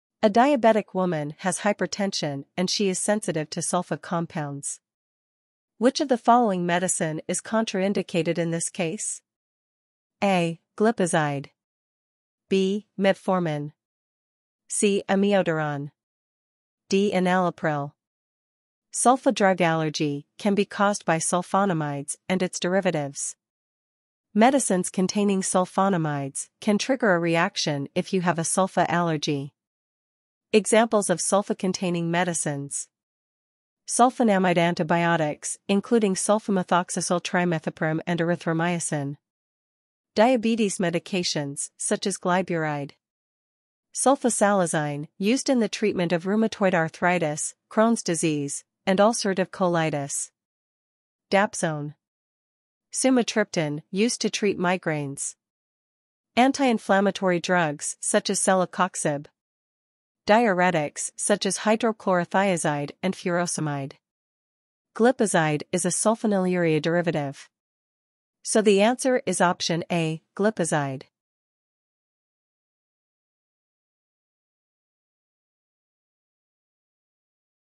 DHA, HAAD PHARMACIST EXAM CASE STUDIES Q&A PART 1
This Q&A series provides case studies and explained answers to the DHA pharmacist exam. It is designed to help pharmacists prepare for the exam and pass it successfully.